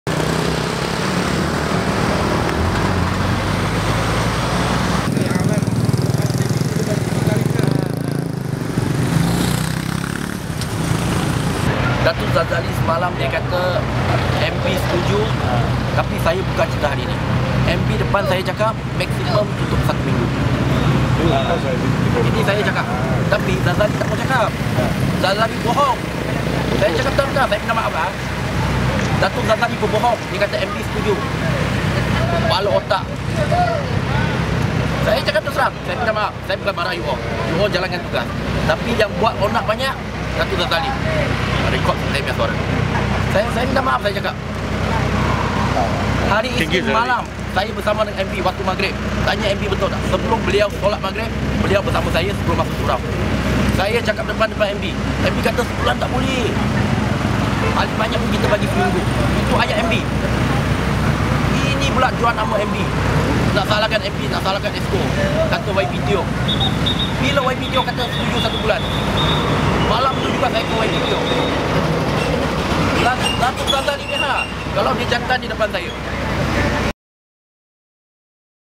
绿峰岭今封路试行 谢琪清：大臣没说过封1个月
芙蓉绿峰岭路口处在一片反对声浪中开始实行局部封堵的试行措施，亚沙区国会议员谢琪清今日把矛头指向市议会主席查扎里，直指“撒谎”，因为州务大臣不曾同意封路一个月！

LINK：

(本报陈梧源摄)